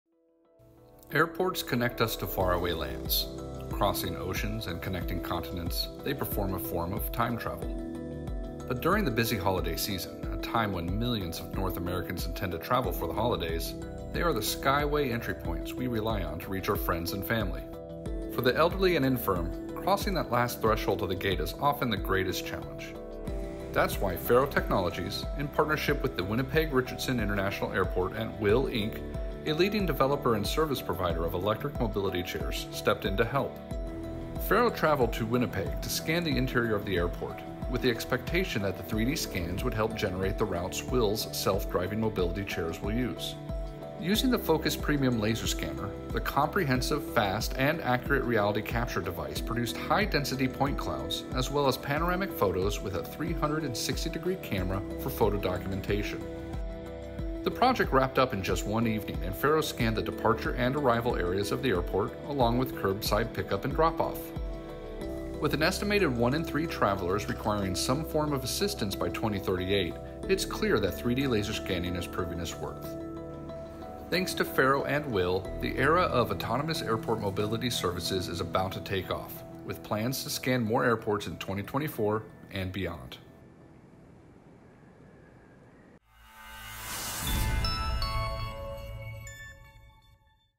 3D Scanning Paves the Way for Autonomous Airport Mobility Services
FARO Technologies joins forces with@ywgairport  and @WHILLUS, a pioneer in electric mobility chairs.

In this video, witness how FARO's cutting-edge Focus Premium Laser Scanner captures the intricate details of the airport interior, revolutionizing accessibility for travelers, especially during the bustling holiday season.

The project's success marks a significant step toward the future of autonomous airport mobility services.

► LEARN MORE about FARO products & innovations